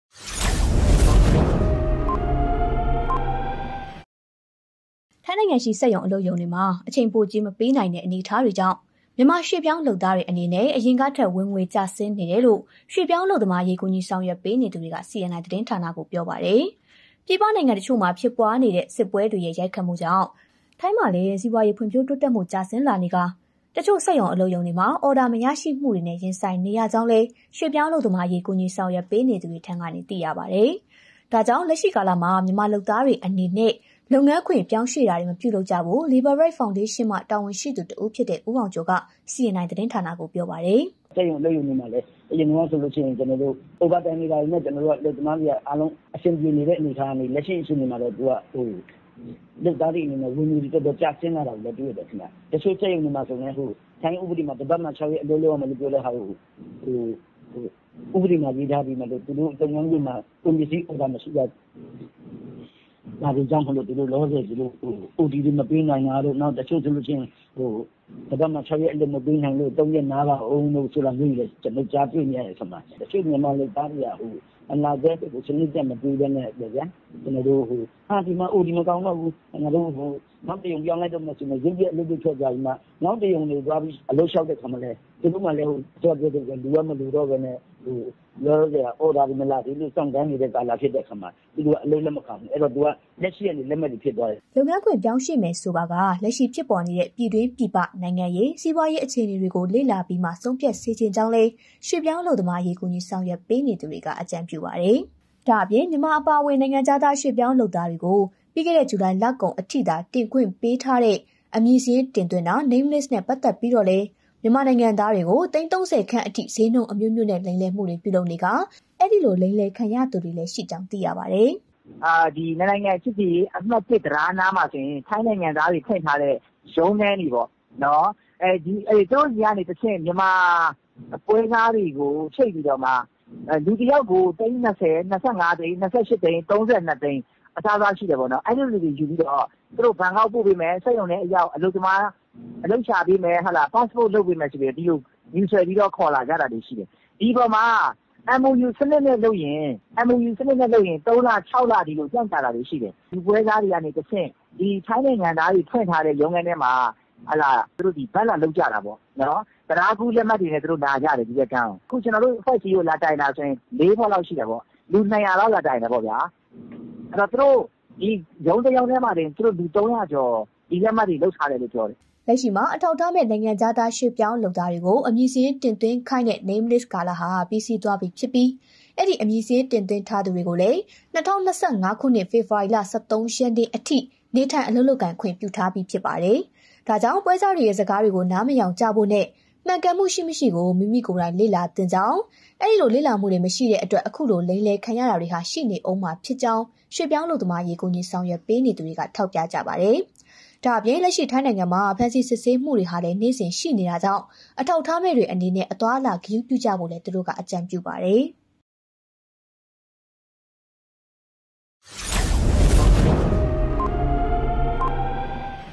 ထိုင်းအလုပ်ရုံများ၌ အချိန်ပိုကြေးမပေးနိုင်၍ မြန်မာလုပ်သားများ ဝင်ငွေကျဆင်း
CNI News

၂၀၂၃ ခုနှစ်၊ အောက်တိုဘာ ၁၇

ထိုင်းနိုင်ငံရှိ စက်ရုံ၊ အလုပ်ရုံများတွင် အချိန်ပိုကြေးမပေးနိုင်သည့်အနေအထားများကြောင့် မြန်မာရွှေ့ပြောင်း လုပ်သားများအနေဖြင့် ယခင်ကထက် ဝင်ငွေကျဆင်းနေကြောင်း ရွှေ့ပြောင်းအလုပ်သမားအရေး ကူညီ ဆောင်ရွက်ပေးနေသူများက CNI သတင်း ဌာနသို့ ပြောသည်။

ပြည်ပနိုင်ငံအချို့၌ ဖြစ်ပွားနေသည့် စစ်ပွဲများ၏ ရိုက်ခတ်မှုကြောင့် ထိုင်းတွင်လည်း စီးပွားရေး ဖွံ့ဖြိုးတိုးတက်မှု ကျဆင်းလာနေကာ အချို့ စက်ရုံ၊ အလုပ်ရုံများတွင် အော်ဒါမရရှိမှုများဖြင့် ရင်ဆိုင်နေရကြောင်း ရွှေ့ပြောင်း အလုပ်သမားအရေး ကူညီဆောင်ရွက်ပေးနေသူများထံက သိရသည်။